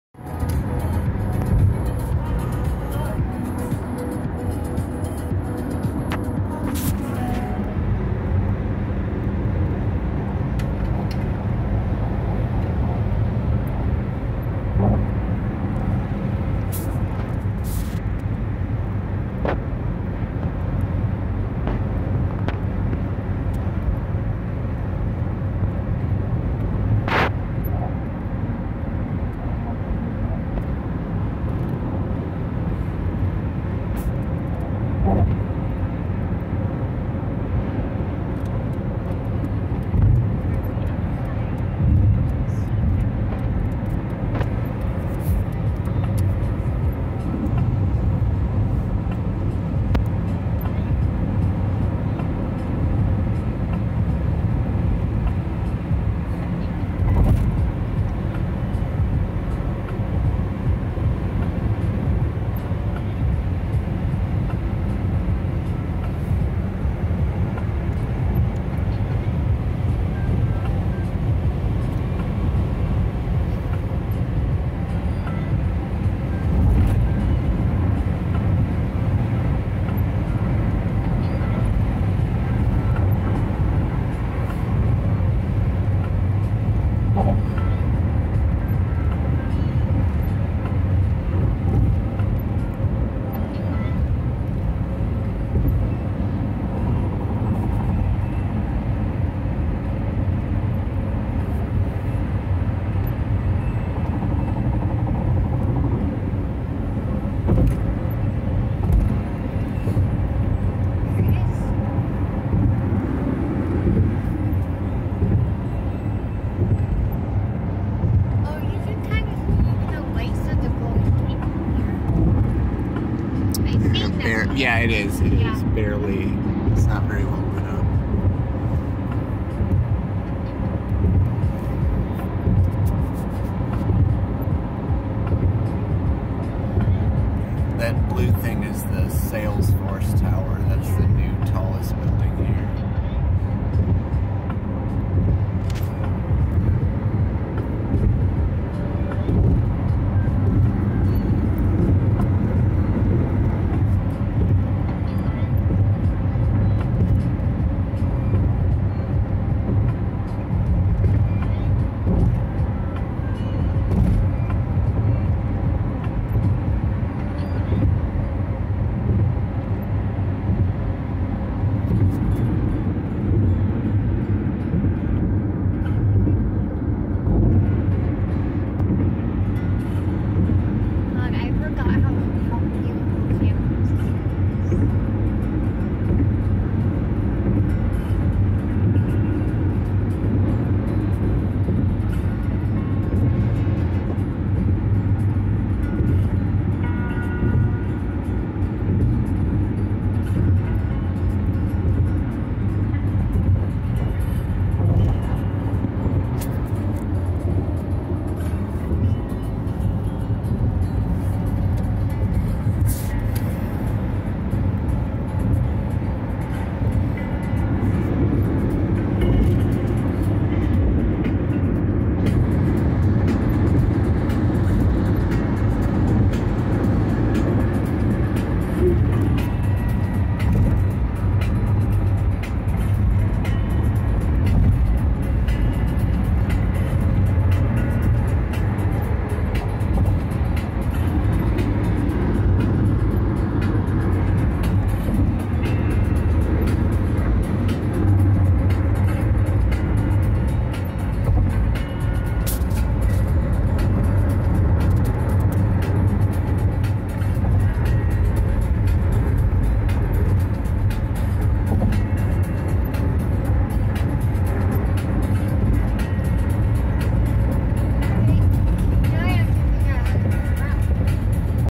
Crossing the SF Bay Bridge at Night
Jakki and I driving across the Bay Bridge from Oakland into downtown San Fransisco, during our trip to the bay area in March.

Please excuse the dirty windshield.